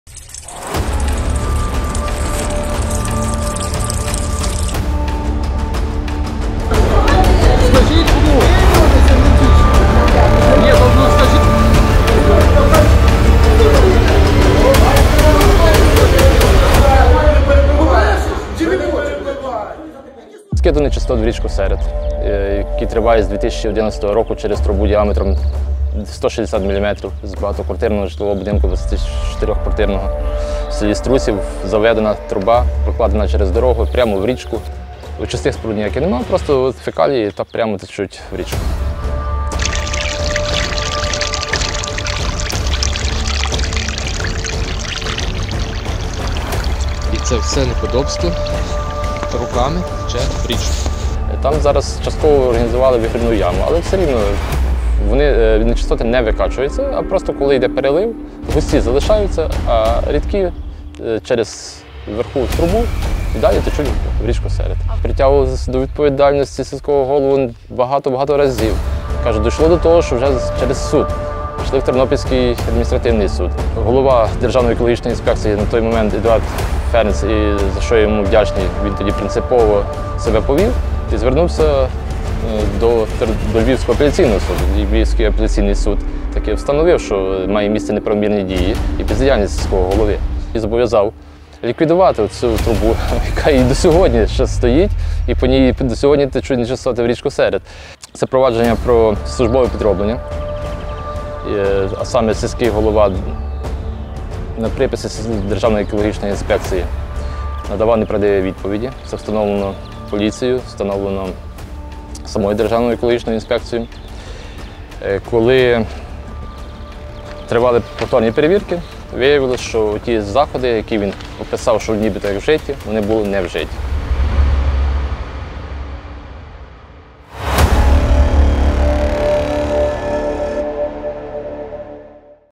Голова Струсівської сільської ради Роман Бойко роками прикриває злив нечистот у річку Серет
Нечистоти з багатоповерхівки у селі Струсів зливають у річку Серет з 2011 року.  Струсівський сільський голова Роман Бойко не те що, «закриває» на це очі, але й вміло прикриває цей екологічний злочин. Після численних скарг та звернень від місцевих жителів забрудненням річки зайнялась екоінспекція.